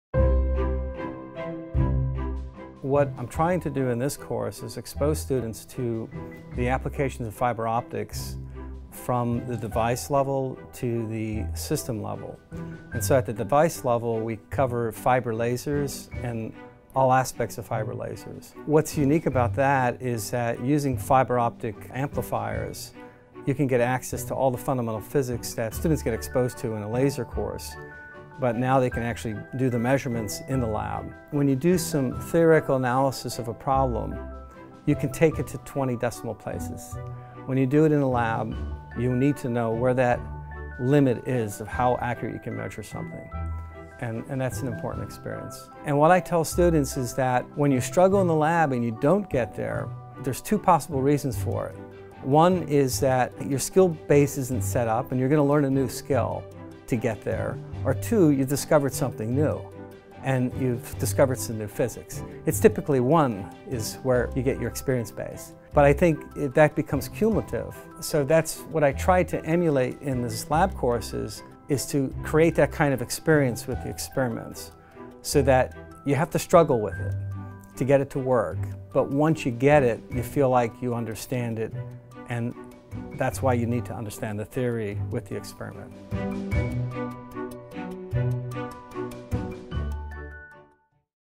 Course Spotlight: Advanced Fiber Optic Laboratory
Taught by popular instructor and laser expert Ray Sova, this hands-on Advanced Fiber Optic Laboratory exposes students to state-of-the-art equipment and fiber optic technologies located at the Johns Hopkins Applied Physics Laboratory. To learn more about our Electrical and Computer Engineering program, please